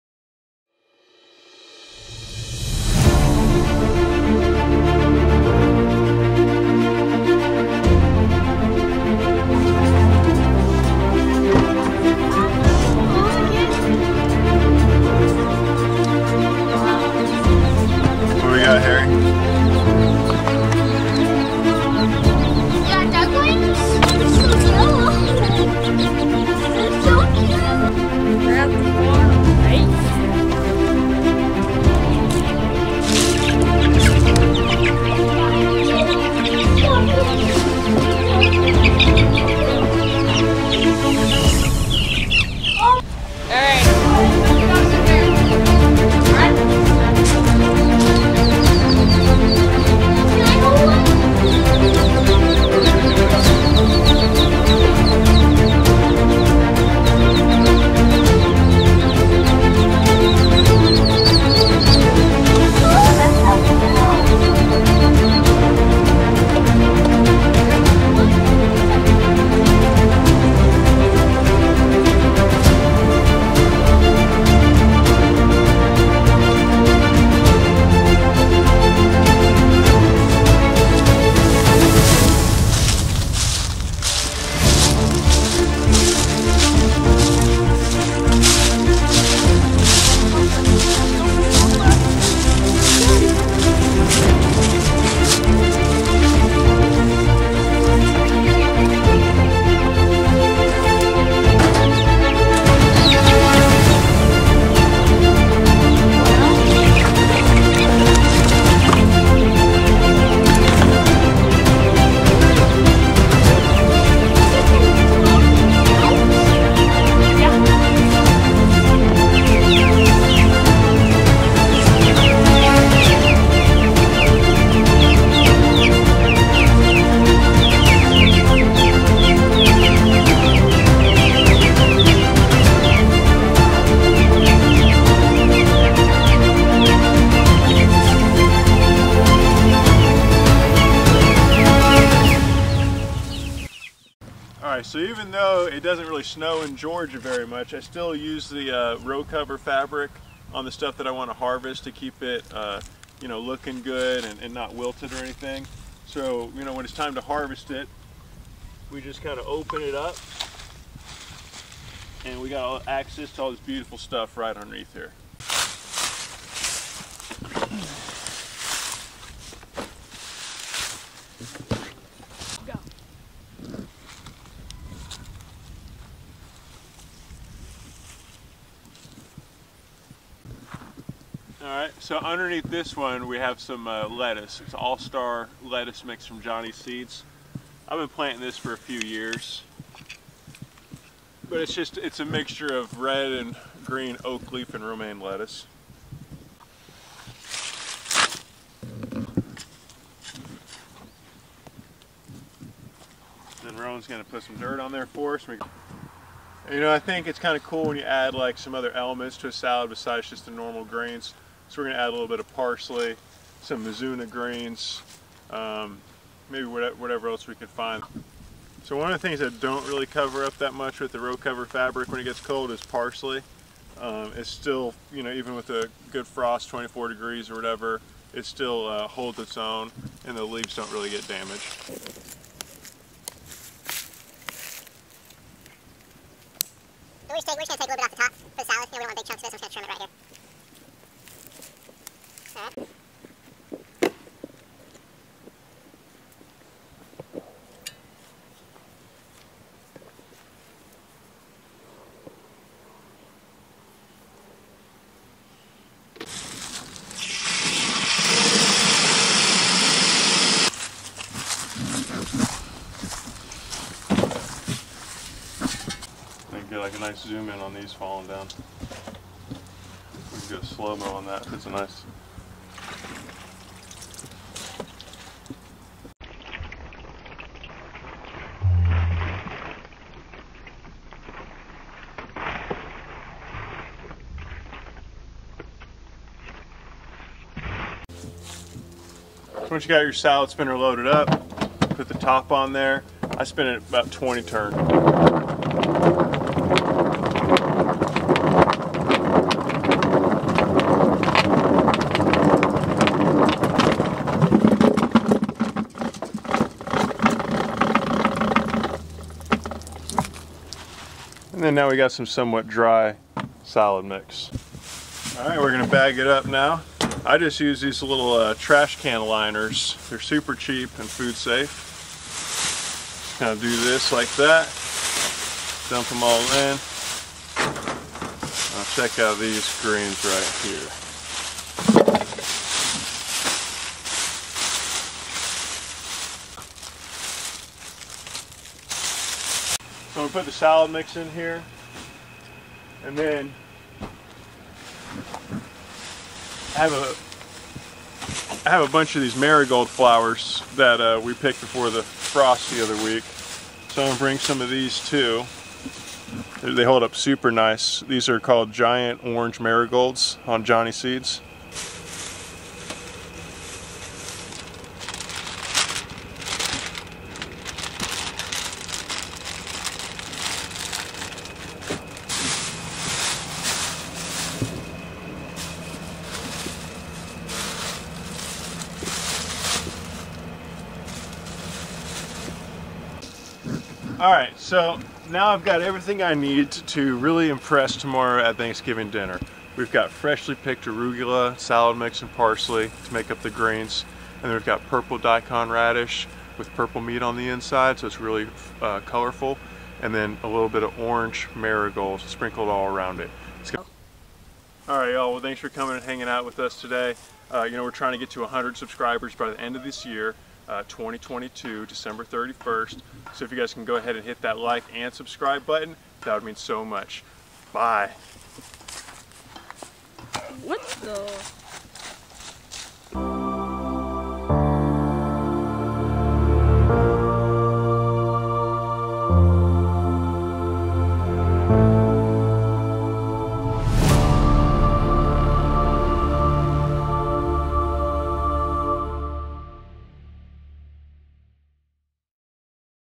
BREMEN FARMS VLOG 001 - NEW Ducklings + Preparing for Thanksgiving
Happy Thanksgiving!!
Ducklings arrived!!
Chicks moved outside

Support our farm!!